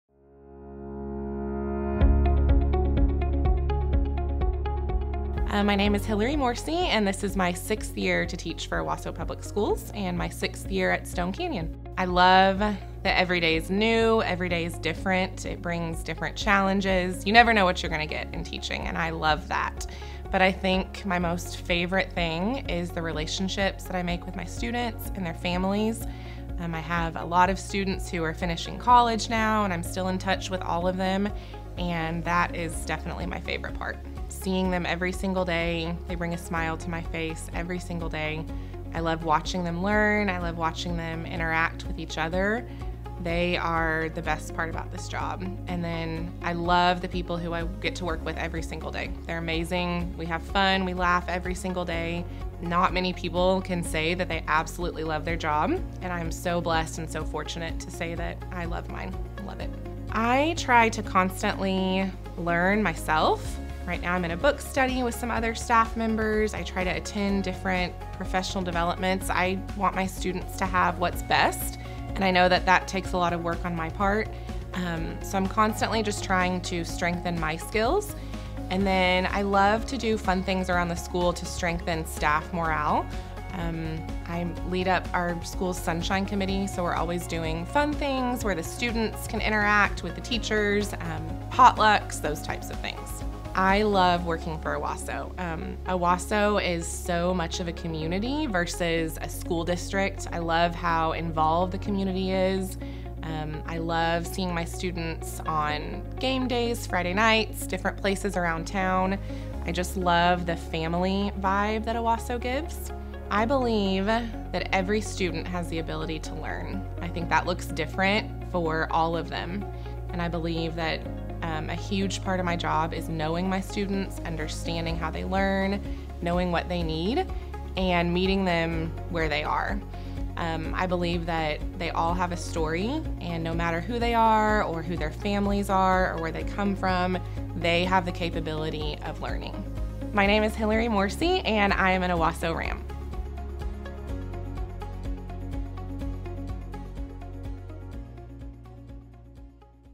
Hilary Morsey - 2023 Stone Canyon Elementary Teacher of the Year
Meet Hilary Morsey, the Teacher of the Year for Stone Canyon Elementary! Mrs. Morsey teaches 3rd grade and she has been in the district for 6 years. Thank you for working hard to provide the best educational experience possible for your students!